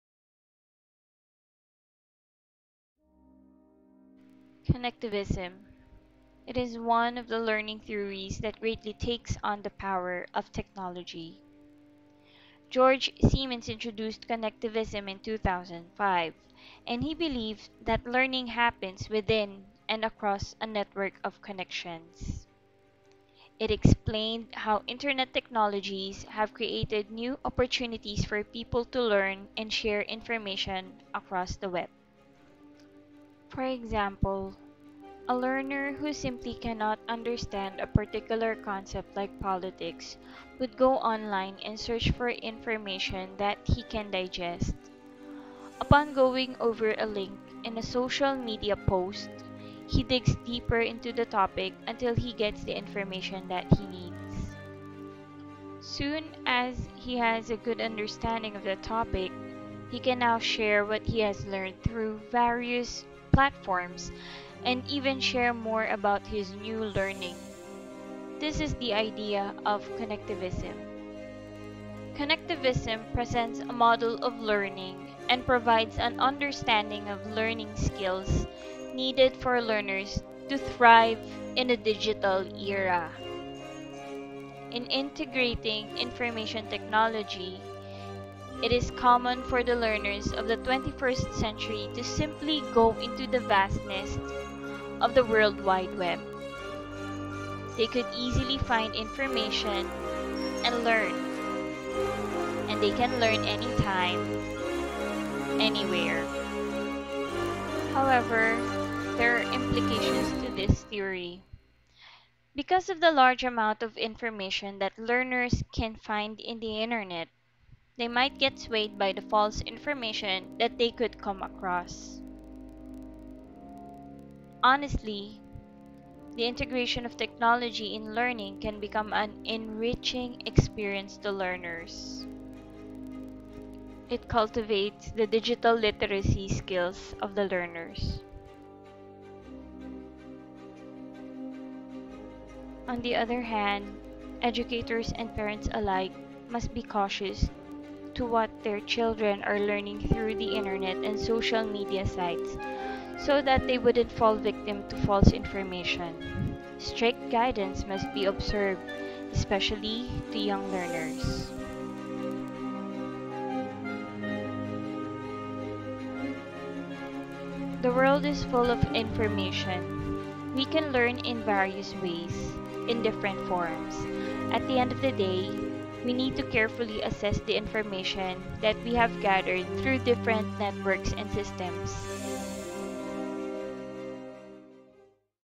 Integrating Technology in Education: Connectivism as a 21st Century Learning Theory
GED 109: Advance Information Technology in Education

This video gives emphasis on the learning theory called Connectivism, and how it works for the 21st Century learners.